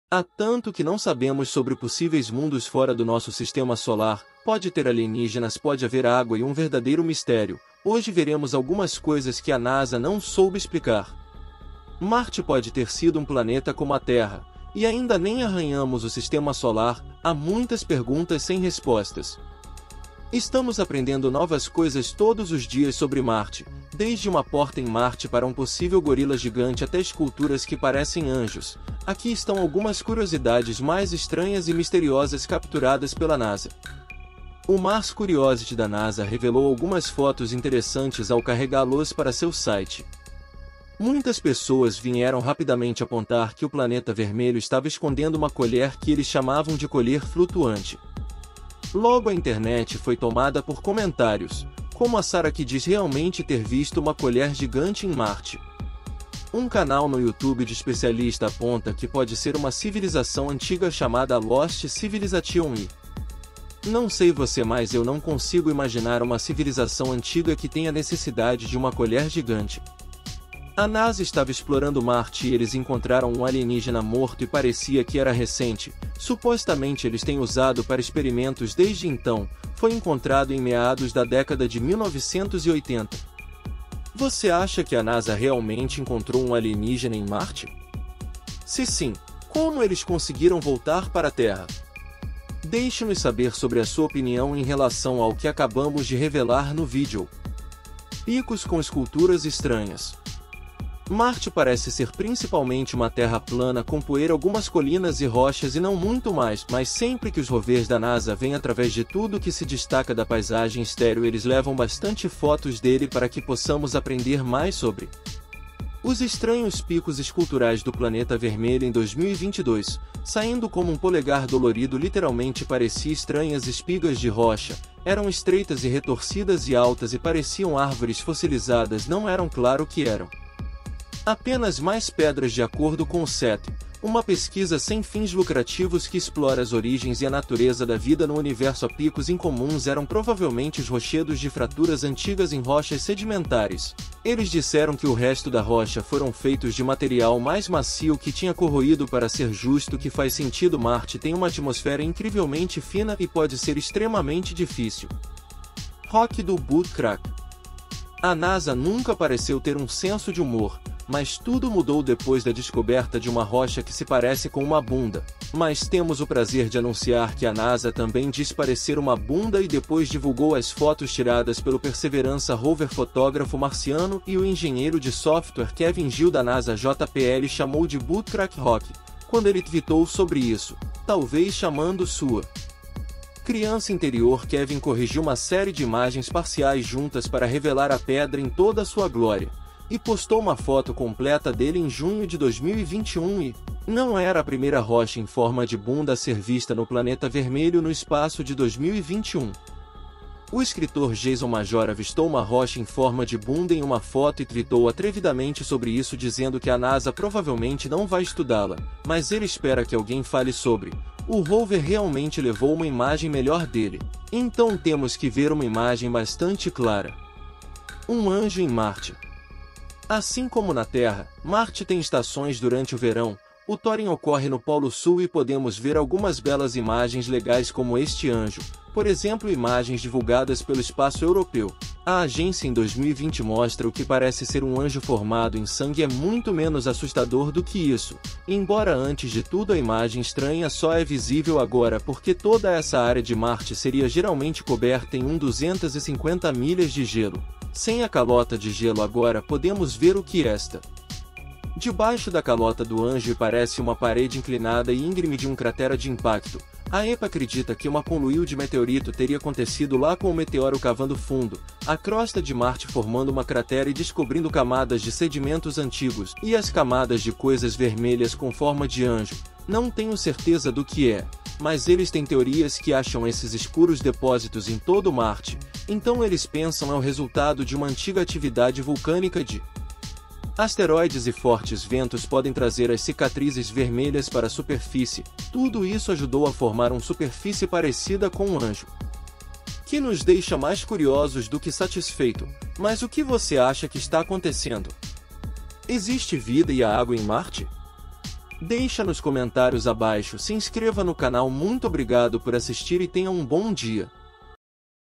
Os especialistas não conseguem explicar o que encontraram em Marte
Os cientistas e pesquisadores têm se deparado com descobertas intrigantes e desconcertantes em Marte, o planeta vermelho. Diversos achados misteriosos e inexplicáveis desafiam a compreensão da comunidade científica. Essas descobertas têm despertado um profundo interesse e curiosidade, levando a teorias fascinantes sobre a possibilidade de vida extraterrestre ou civilizações antigas no planeta vizinho.

Desde formações rochosas intrigantes, padrões geológicos peculiares, até objetos que aparentam ser estruturas ou artefatos, o mistério só aumenta. A análise cuidadosa das imagens e dados coletados pelas sondas e rovers enviados a Marte revela pistas enigmáticas que desafiam as explicações convencionais.

Embora haja uma intensa busca por respostas, muitas das descobertas permanecem sem uma explicação clara. As teorias variam desde processos geológicos naturais até evidências de antigos assentamentos extraterrestres. O enigma em torno dessas descobertas continua a intrigar e estimular a imaginação das pessoas ao redor do mundo.